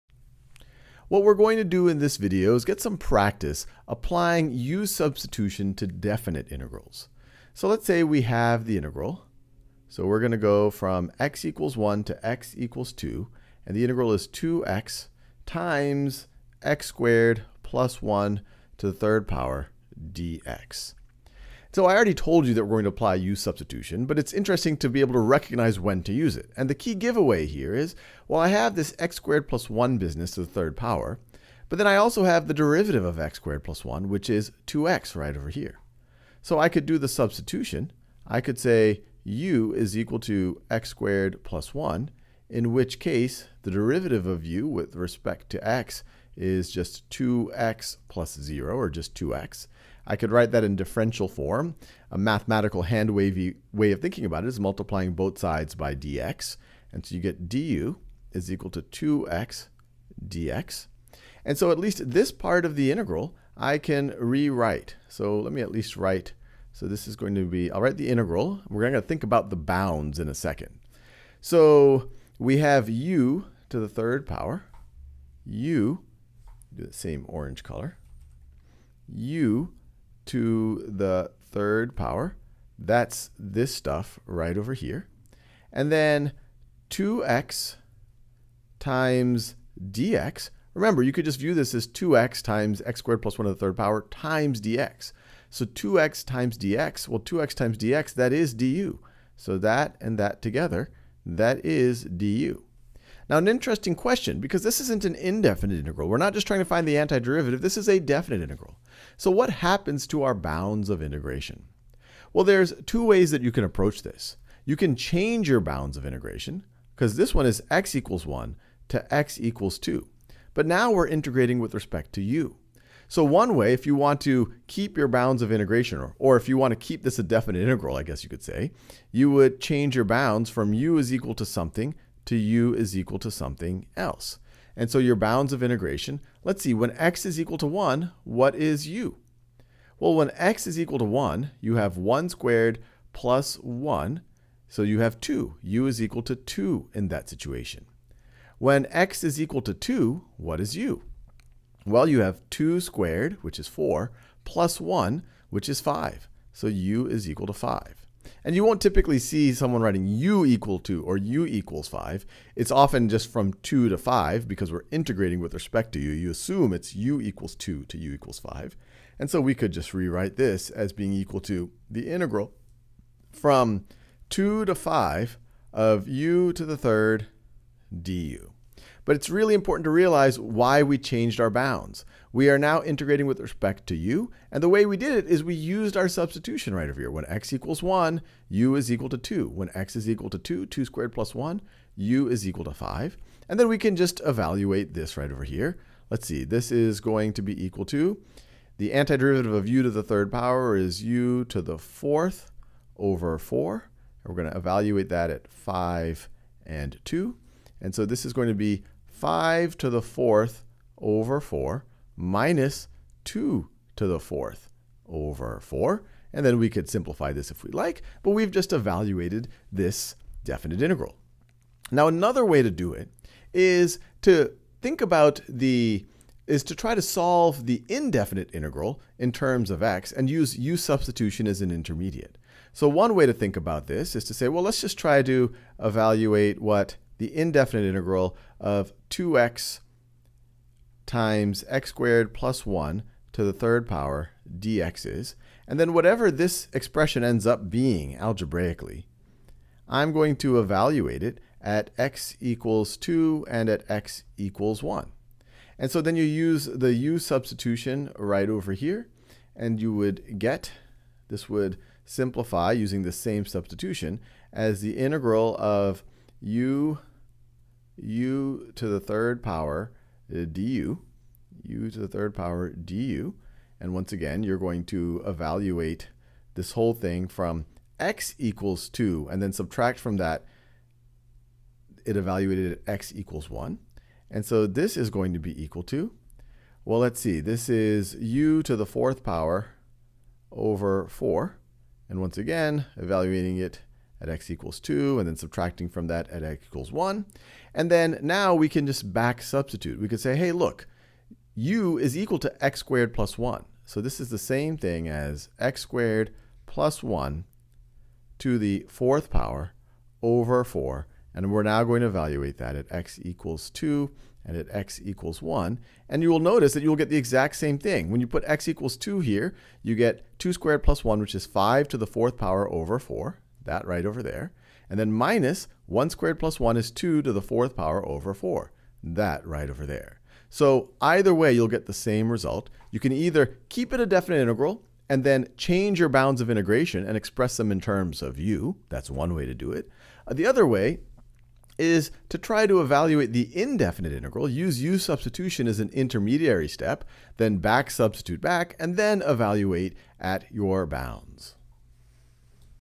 _-substitution: definite integrals | AP Calculus AB | Khan Academy
Courses on Khan Academy are always 100% free. Start practicing—and saving your progress—now

When using _-substitution in definite integrals, we must make sure we take care of the boundaries of integration.